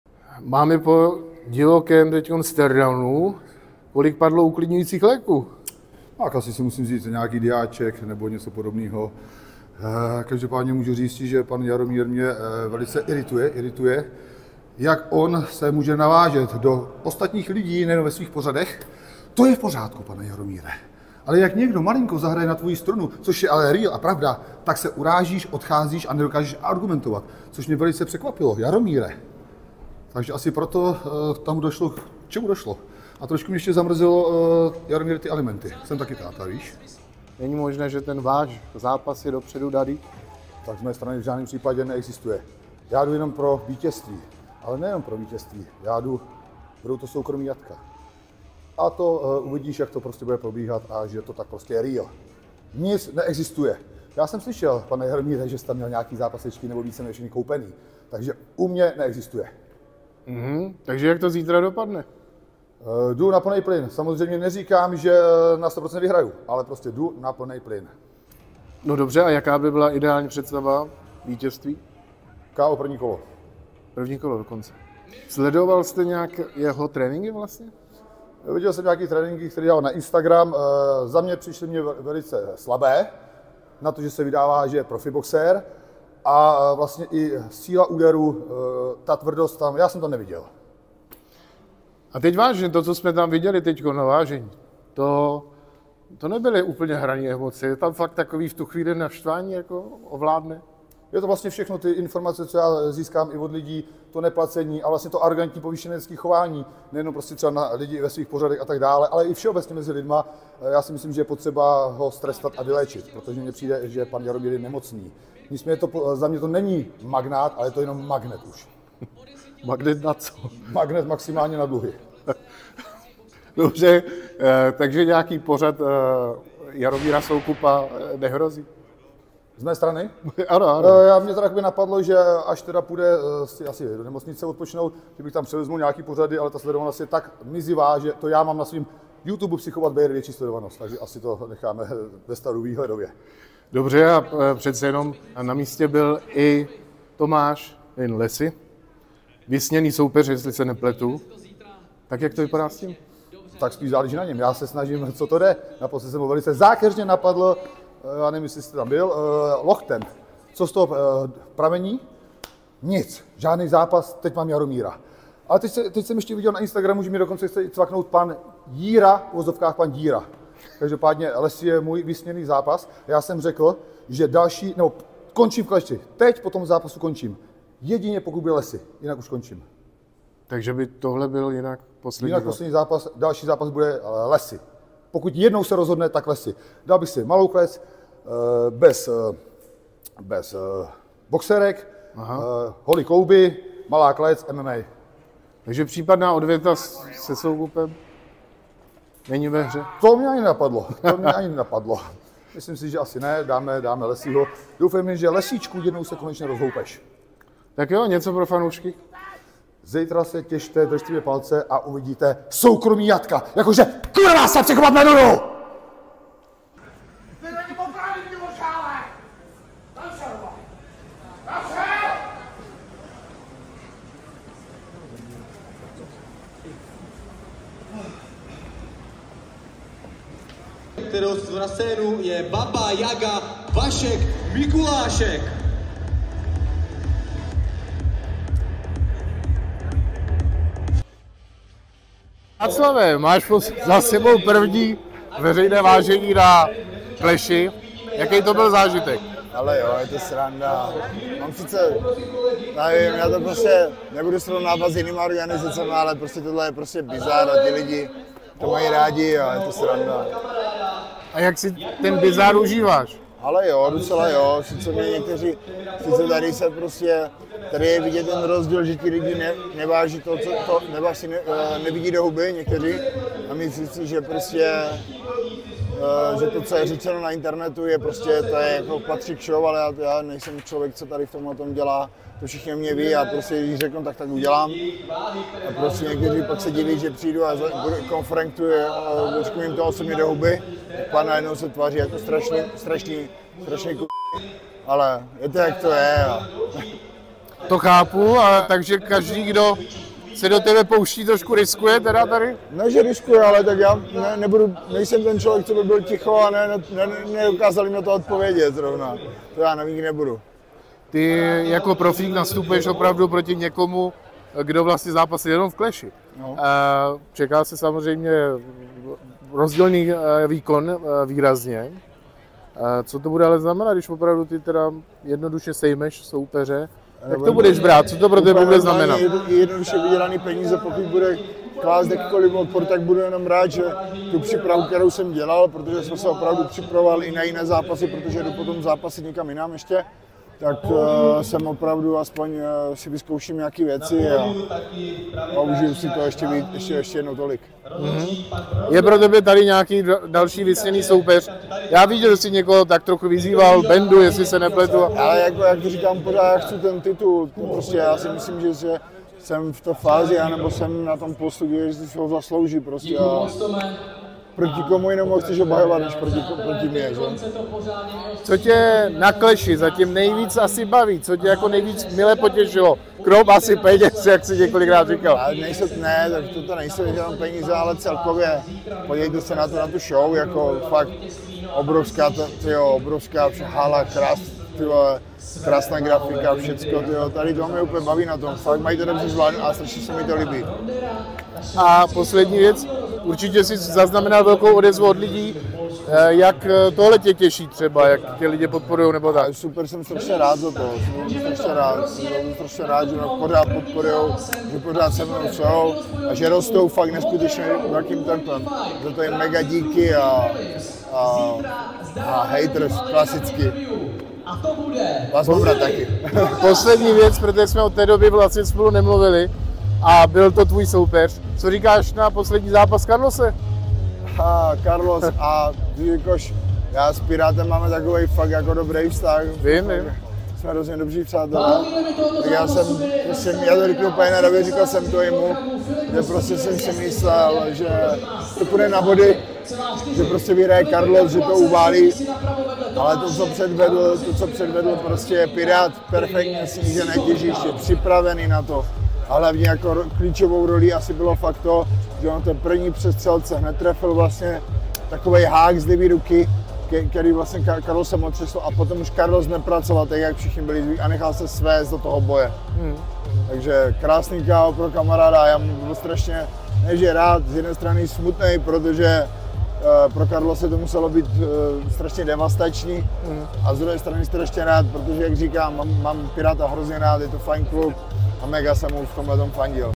Clash of the Stars 7: Bijec Bejr slibuje Jaromíru Soukupovi „soukromá jatka“
Emoce před sobotním zápasem mezi šéfem TV Barrandov Jaromírem Soukupem a influencerem Alešem „Psychopatem“ Bejrem na turnaji Clash of Stars gradují. Bejr svému soupeři v rozhovoru pro CNN Prima NEWS slibuje „soukromá jatka“. Soukup se na výzvu těší.